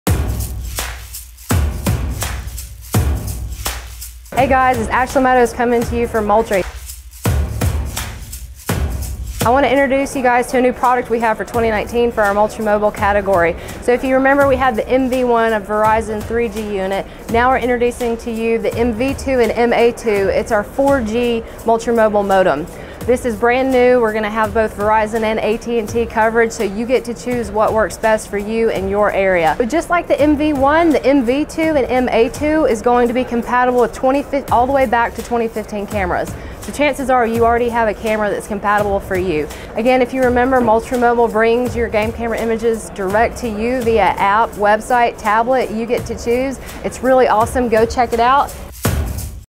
Moultrie Mobile MV2 & MA2 4G Cellular Field Modems
Moultrie’s MV2 and MA2 Cellular Field Modems quickly and simply convert any Moultrie game camera made since 2015 into a wireless game camera. Operating on the Verizon (MV2) and AT&T (MA2) 4G cellular networks, the modems transmit images and videos directly to the industry-leading and user-friendly Moultrie Mobile app.